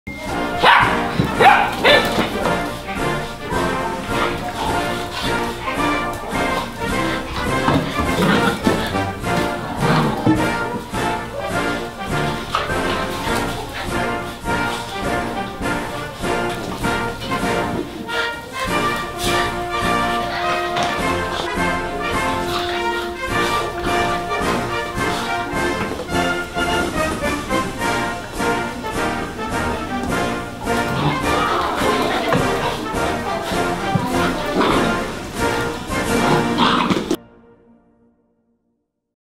Great Plains SPCA: Puppies not Politics!
There's nothing quite as paw-triotic as puppies. We promise it won't offend your grandma.